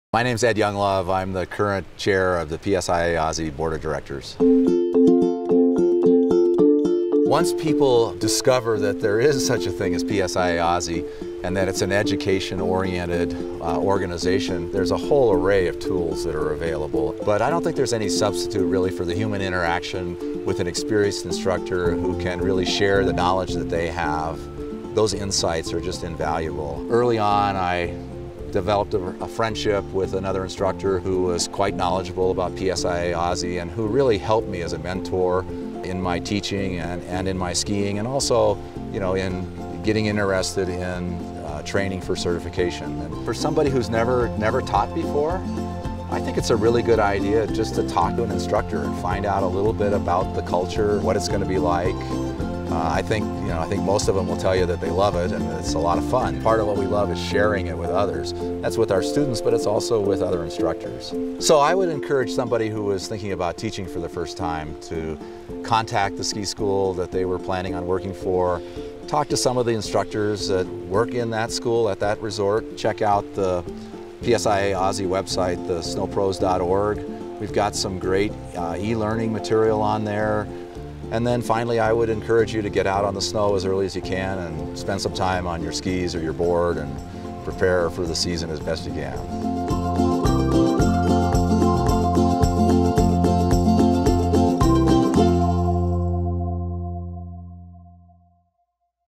How to Work as a Ski or Snowboard Instructor
PSIA-AASI Board Chair Ed Younglove offers words of wisdom for new and future instructors – including the value of interacting with more experienced colleagues and taking advantage of PSIA-AASI’s education tools.